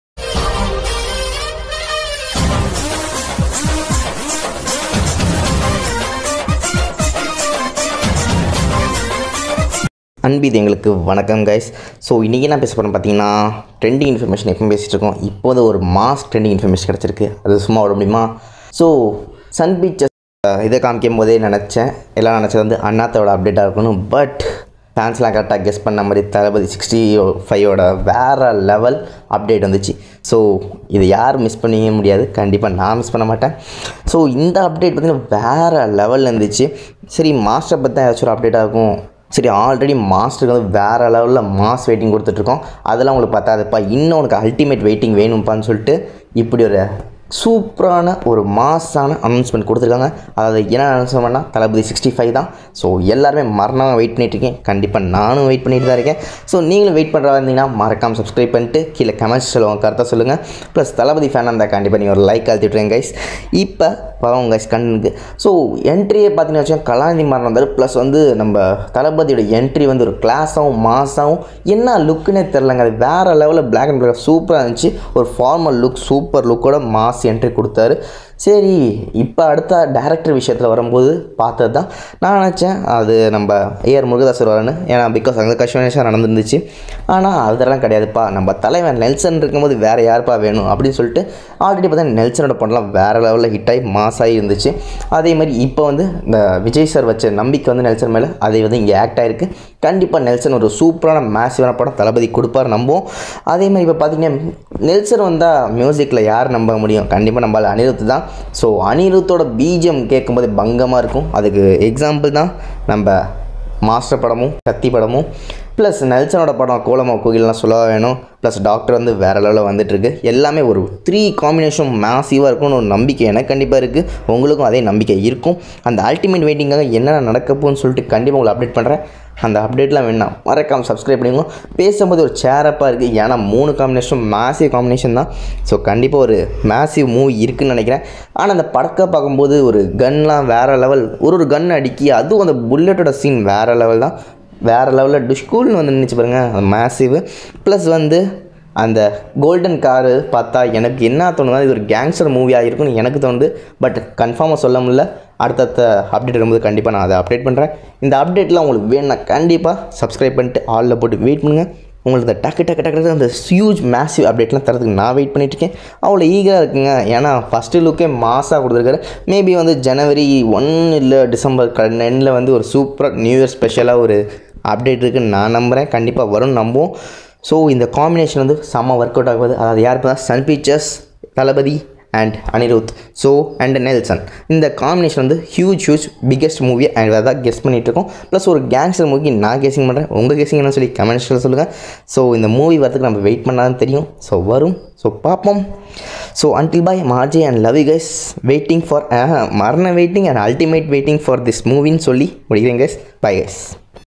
Thalapathy 65 mass update | thalapathy fans special | marana waiting for movie
Yesterday the official announcement for thalapathy 65 is given and updated and first look information also is provided and special update for fans also given..